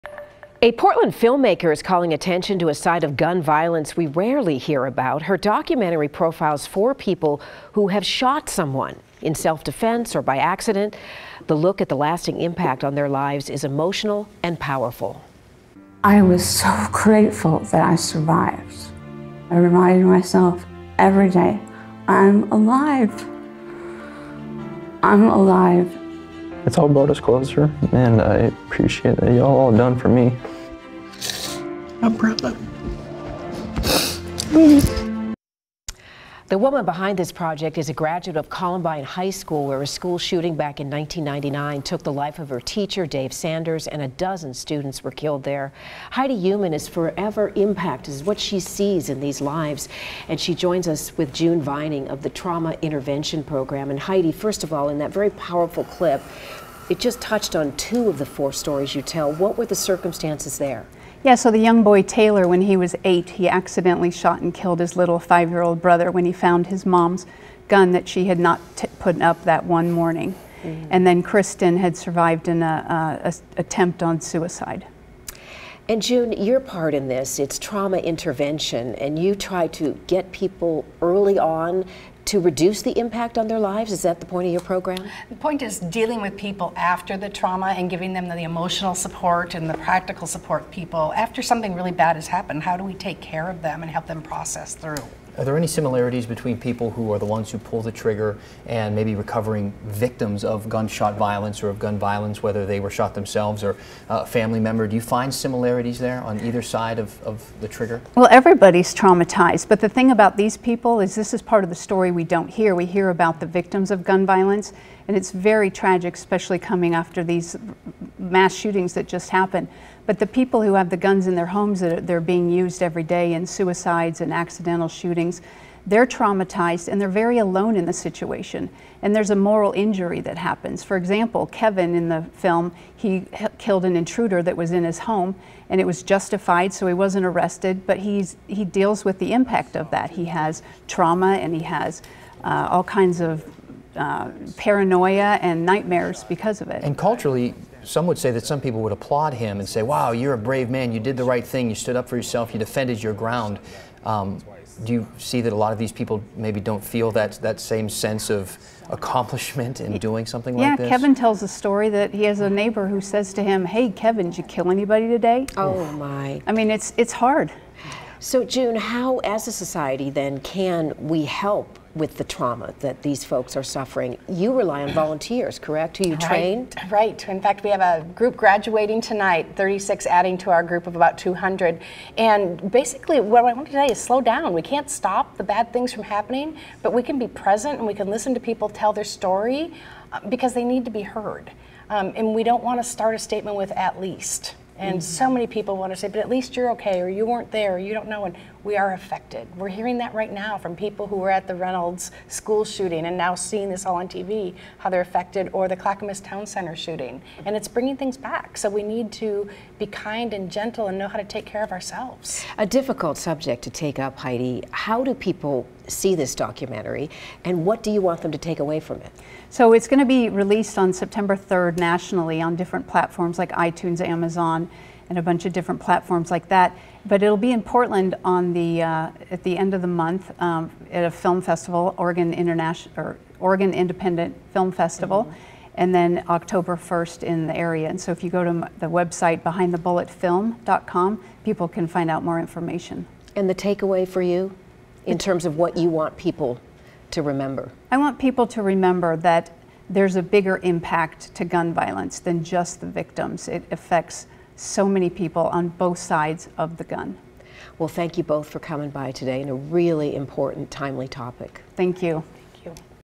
Portland filmmaker’s documentary explores other side of gun violence
“Behind the Bullet” looks at the impact shootings have on the shooters themselves.